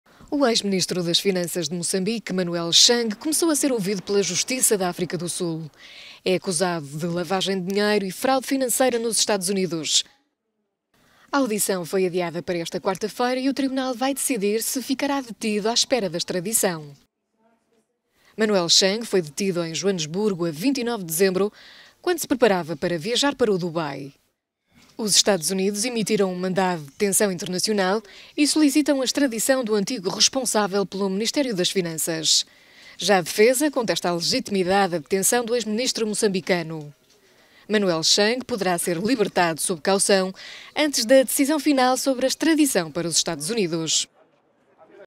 Antigo ministro das finanças de Moçambique ouvido em tribunal
É acusado de lavagem de dinheiro e fraude financeira nos Estados Unidos.…